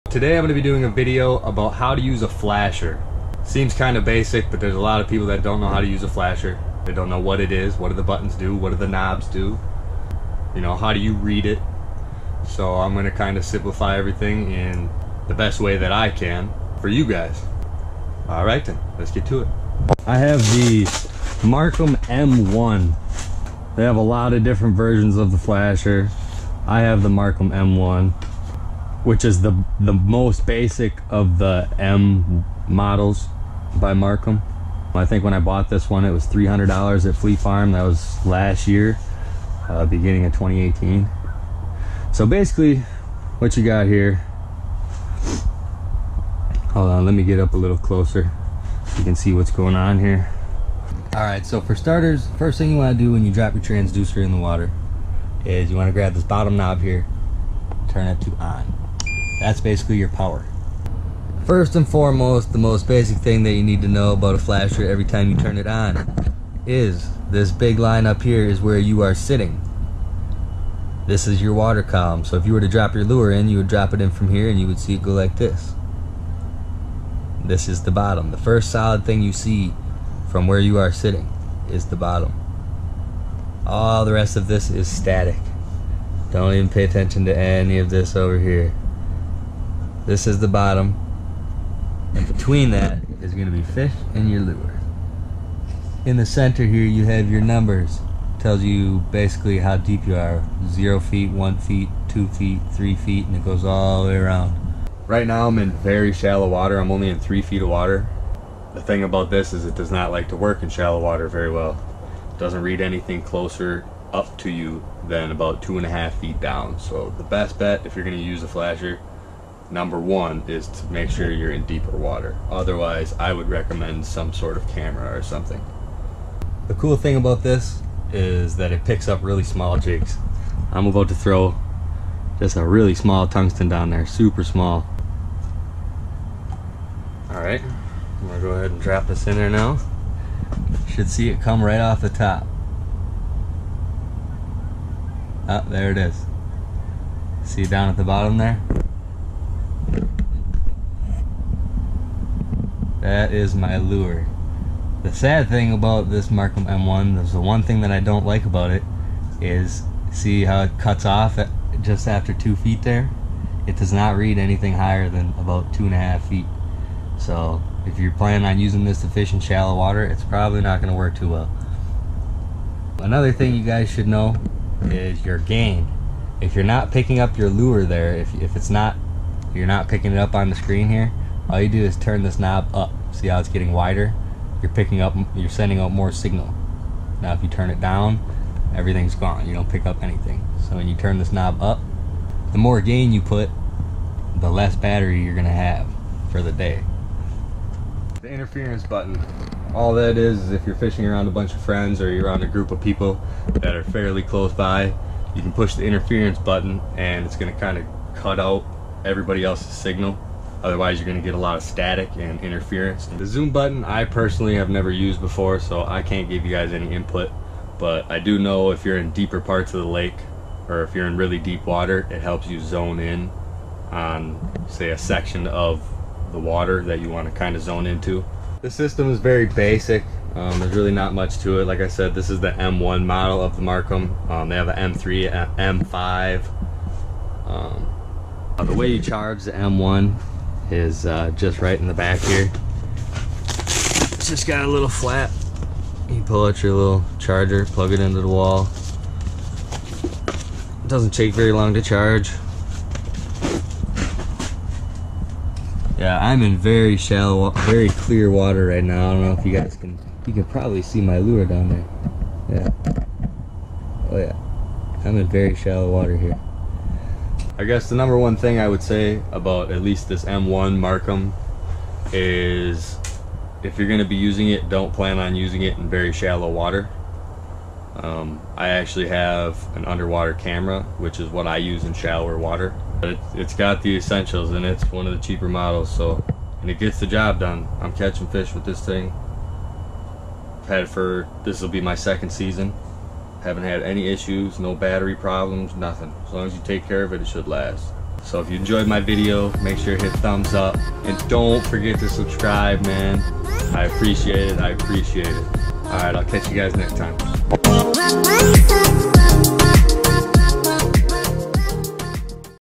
How To Read a Flasher | Marcum M1 Review | Ice Fishing
- In this beginners video i explain the basics of how to read and how to use a flasher for ice fishing!

- Product Reviewed: Marcum M1 Flasher

Follow me